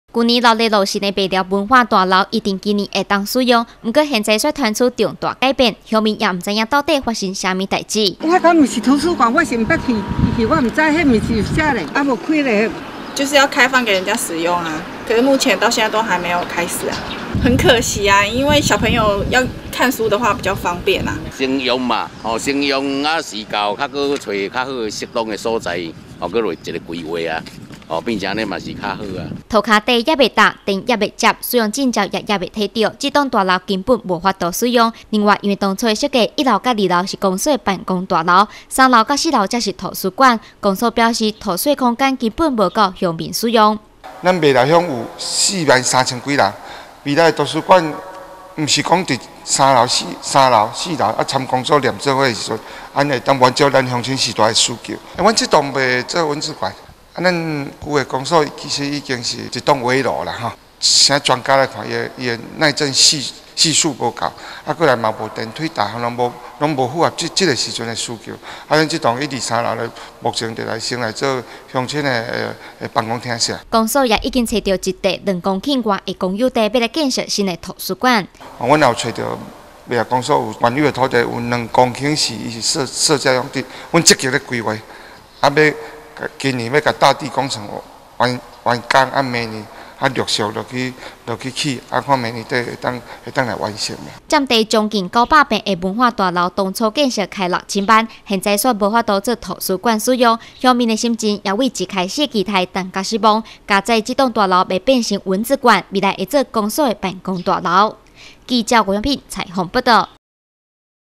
雲林新聞網-麥寮文化大樓遲未開放民眾期望落空
去年十二月才熱鬧辦理落成典禮的麥寮文化大樓，本預定設計成圖書館，現在卻驚傳重大改變，麥寮鄉長許忠富表示，原本設計一、二樓是辦公大樓，三樓四樓才是圖書館，只有兩層樓的圖書空間根本不夠讓鄉民使用，所以他們重新找到一塊兩公頃的公有地，要來建設一棟多功能圖書館，現在的文化大樓未來計劃作麥寮鄉公所的辦公大樓。